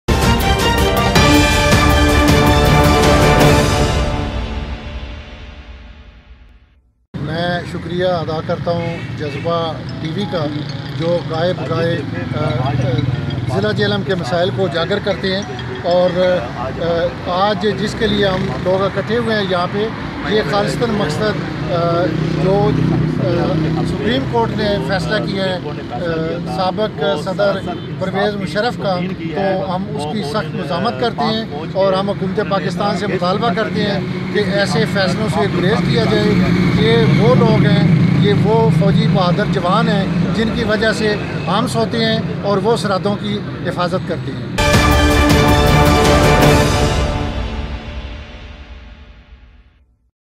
President Jhelum Press club Ch Abid Talking About Jazba Tv
Raheel Khalil ,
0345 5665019